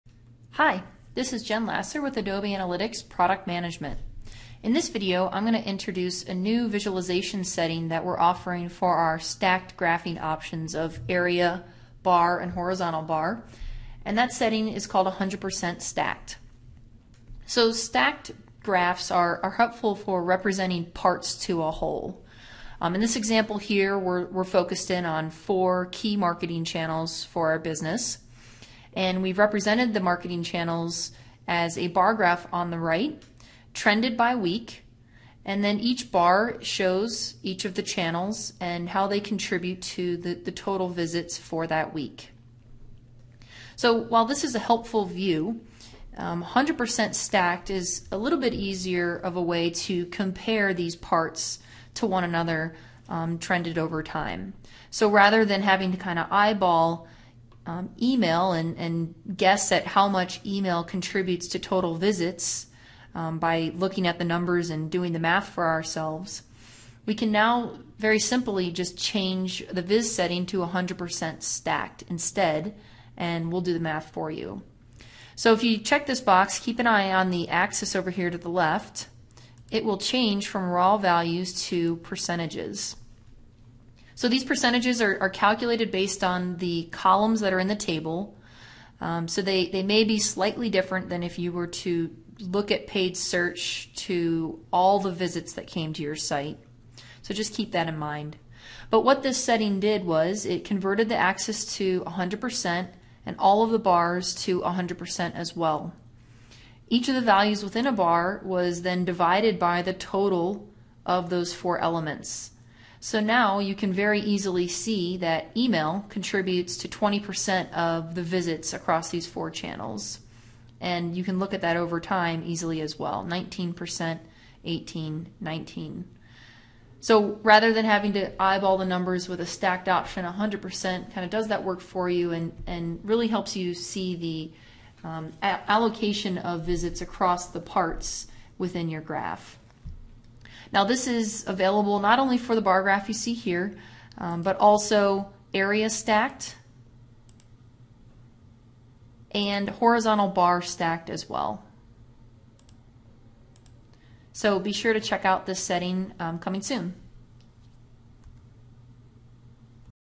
100% Stacked Visualizations in Analysis Workspace
Stacked graphs are helpful for representing parts to a whole in a trend. The new 100% stacked viz setting further helps to quickly see how each part contributes to the whole, in terms of a % value instead of a raw value. This new setting is available from the Visualization Settings gear in the top right of Area Stacked, Bar Stacked and Horizontal Bar Stacked vizs.